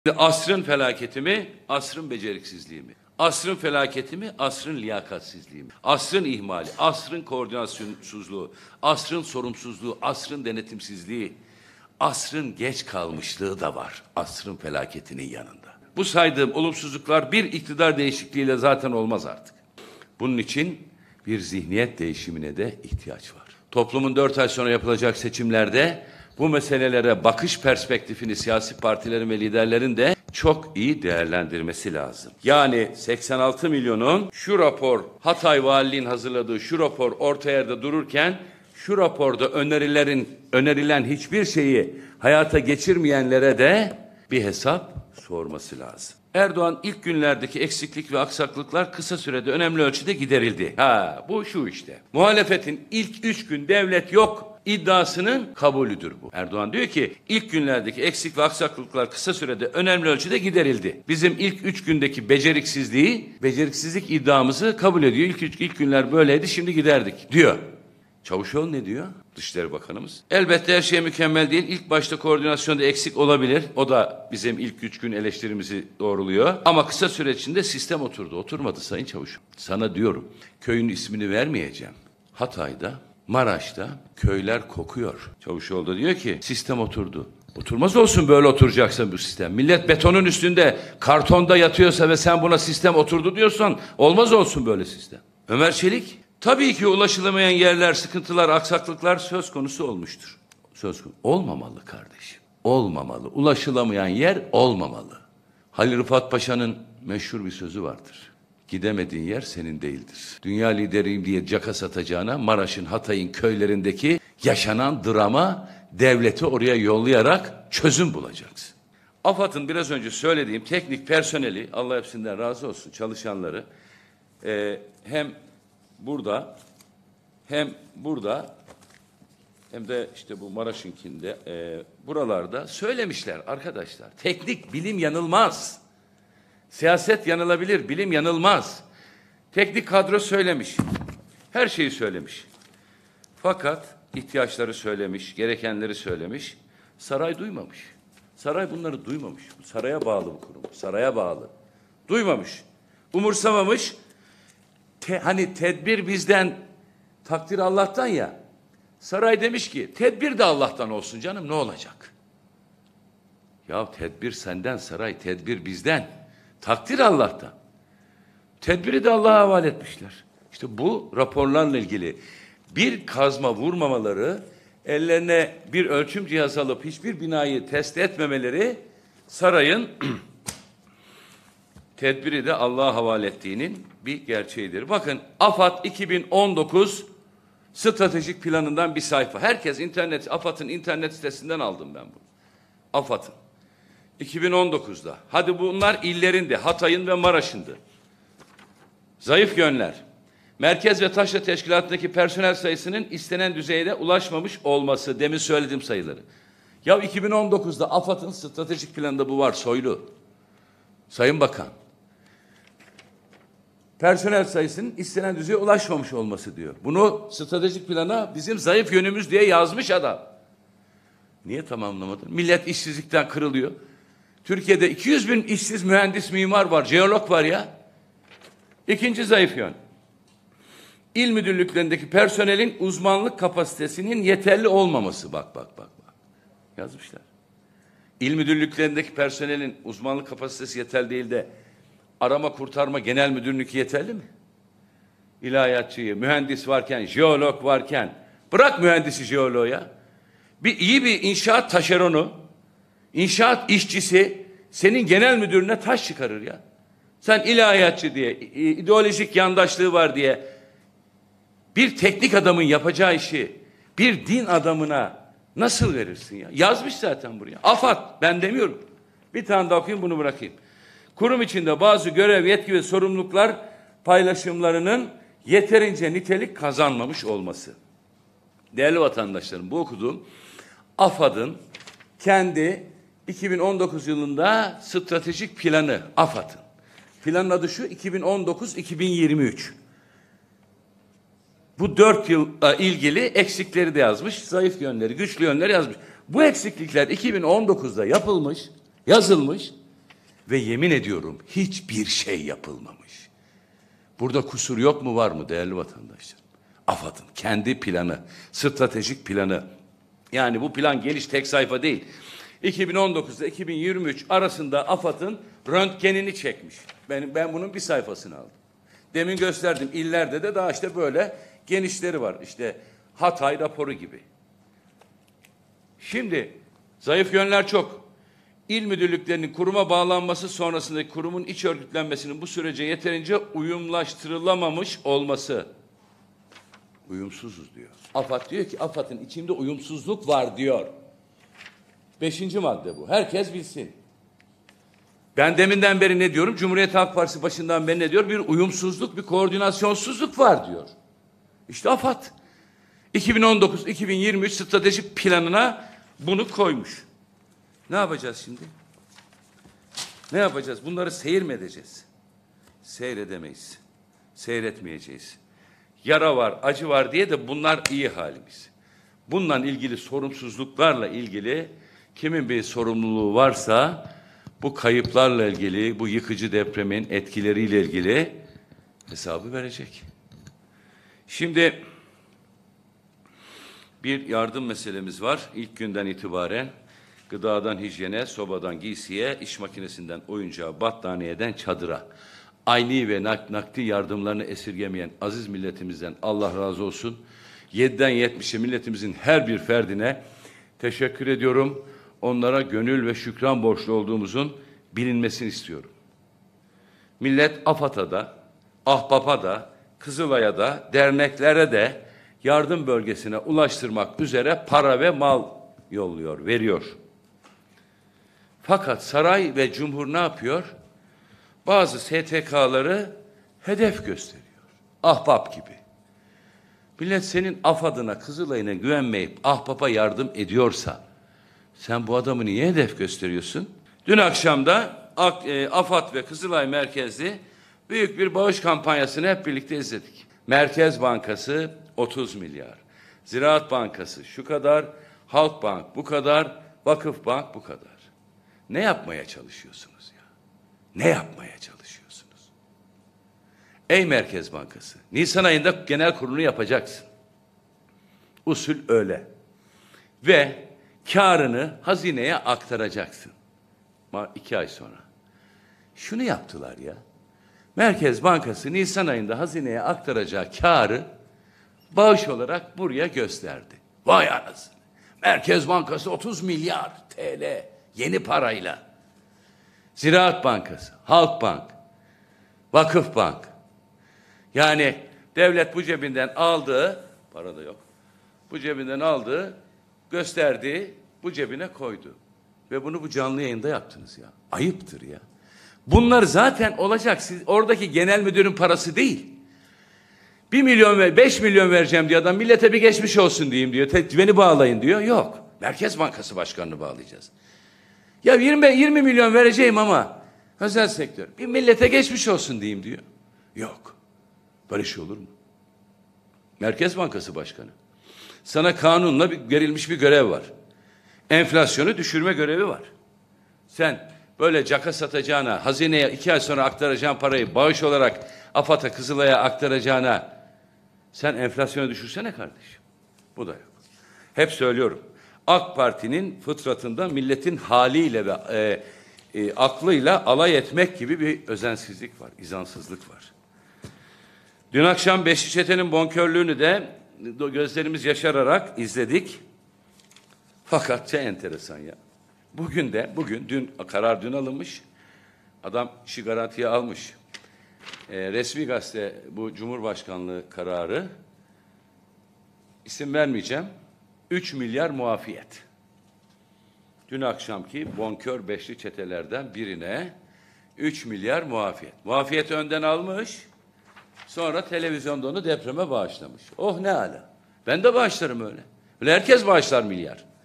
Engin Altay Erdoğan ve Soylu'yu zora sokacak o raporu açığa çıkardı!
CHP Grup Başkanvekili Engin Altay TBMM'de düzenlediği basın toplantısında AFAD'ın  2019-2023 yıları stratejik plan raporunu gözler önüne serdi.